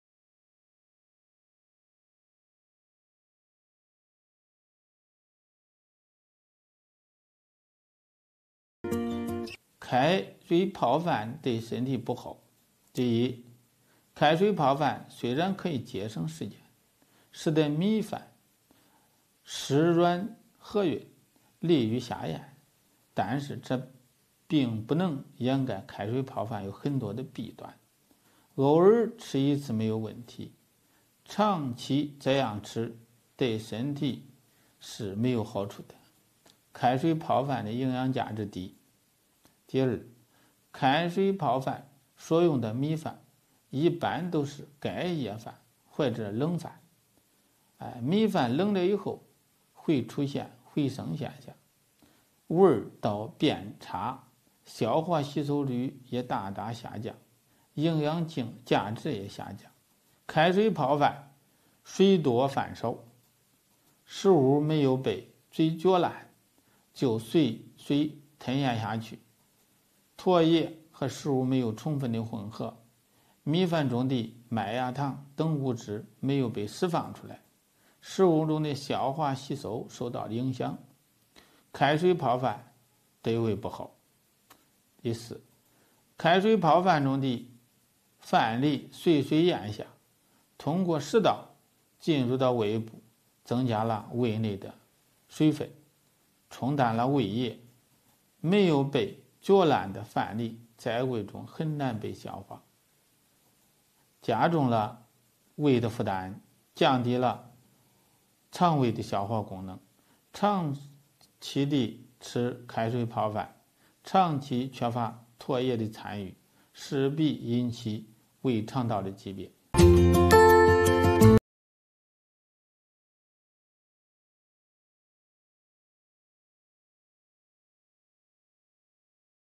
开水泡饭好吗？ 续海卿 河南中医药大学第三附属医院
开后水泡饭对身体不好，第一开水泡饭虽然可以节省时间使得米饭湿软和润利于下咽，但是这并不能掩盖开水泡饭很多的弊端，偶尔吃一次没有问题长期这样吃对身体是没有好处的，开水泡饭营养价值低。第二开水泡饭所用的米饭一般都是隔夜饭或者冷饭，米饭冷了之后会出现回生现象味道变差，消化吸收率也大大下降营养价值也下降，开水泡饭水多饭少食物没有被嚼烂就随水吞咽下去，唾液和食物没有充分的混合米饭中的麦芽糖等物质，没有被释放出来食物中的消化吸收受到影响，开水泡饭对胃不好。第四开水泡饭中的饭粒随水咽下通过食道进入到胃部增加了胃内的水分冲淡了的胃液，没有被嚼烂的饭粒在胃中很难被消化加重了胃的负担，降低了肠胃的消化功能，长期的吃开水泡饭、长期缺乏唾液的参与势必引起胃肠道的疾病。

How about boiled rice?
It's not good for your health to have a bubble rice, Although it can save time to make rice wet, soft and moist for swallowing, But it can't cover up many disadvantages of boiled rice, It's no problem to eat once in a while. It's not good for your health for a long time, The nutritional value of boiled rice is low. The rice for the second boiled rice is usually overnight or cold, When the rice is cold, it will come back to life, The digestibility and absorptivity are also greatly reduced, and the nutritional value is also reduced, Boiled water makes rice more, rice less, food is not chewed and swallowed with water, Saliva and food do not mix maltose and other substances in rice, The digestion and absorption of food not released are affected, Boiled rice is bad for the stomach. The rice grains in the fourth water soaked rice enter into the stomach through the esophagus after swallowing, which increases the water in the stomach and dilutes the gastric juice, It is very difficult to digest the rice grains without being chewed in the stomach, which increases the burden on the stomach, It reduces the digestive function of the intestine and stomach, Eating boiled water for a long time and lacking saliva for a long time are bound to cause gastrointestinal diseases.